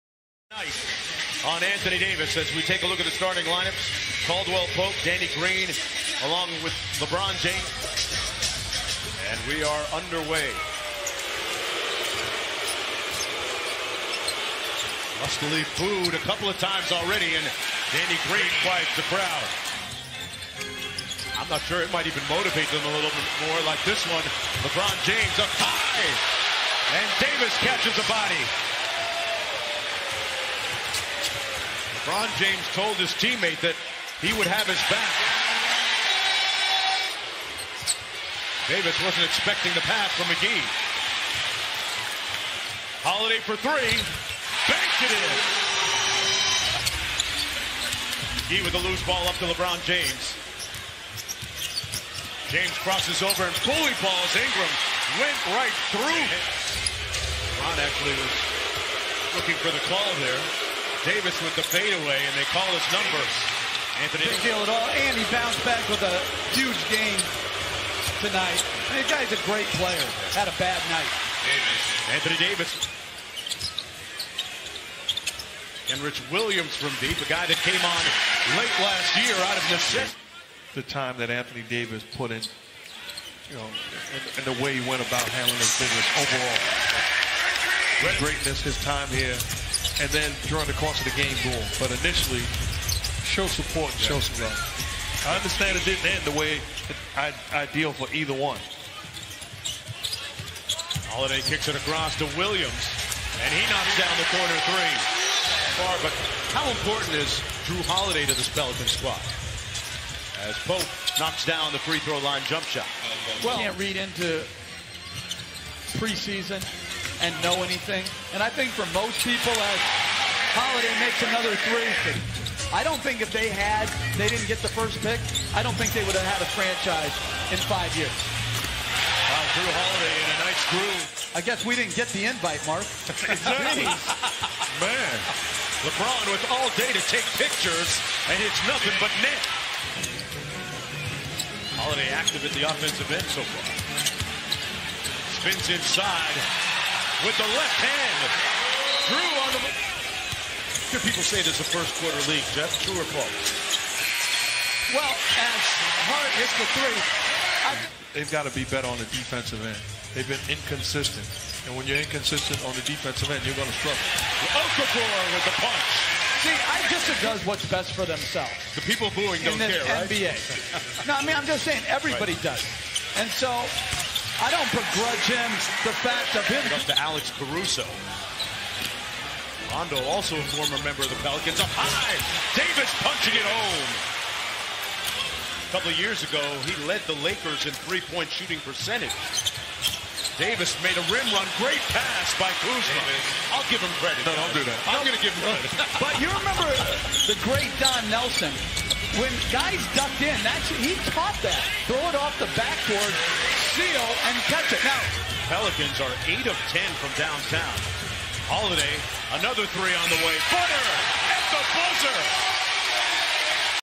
Los Angeles Lakers vs New Orleans Pelicans - 1st Qtr Highlights | Nov 27, 2019-20 NBA Season
Follow my new media channel Los Angeles Lakers vs New Orleans Pelicans - 1st Qtr Highlights | Nov 27, 2019-20 NBA Season